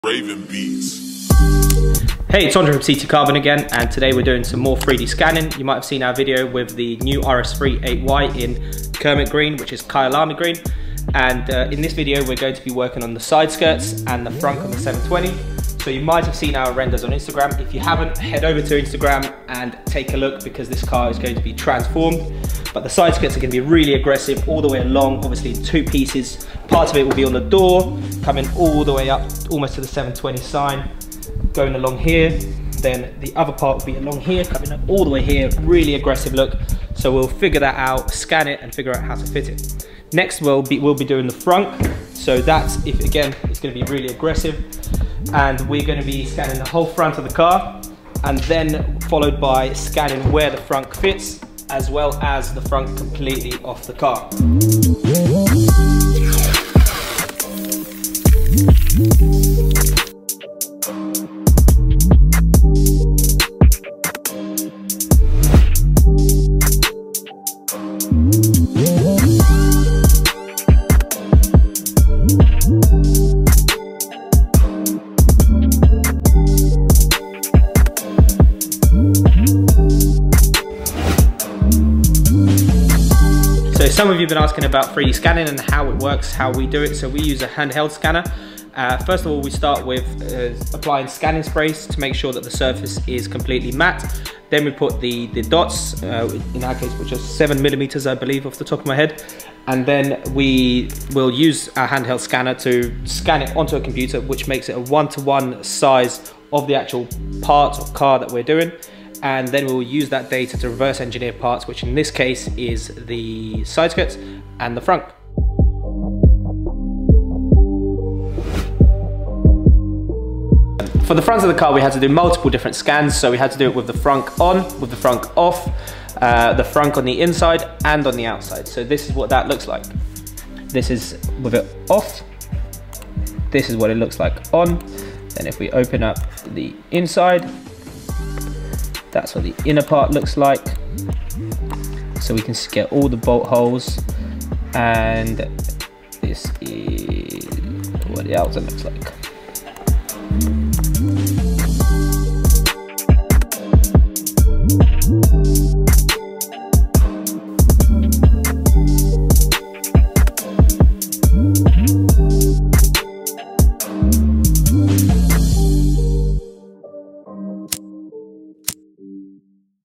CT Carbon | McLaren 720s | 3D Scanning Carbon Frunk & Side Skirt
Continuing with our work on the McLaren 720s we show you how we 3D scan the frunk and the side skirts. Super excited to show you guys the final product!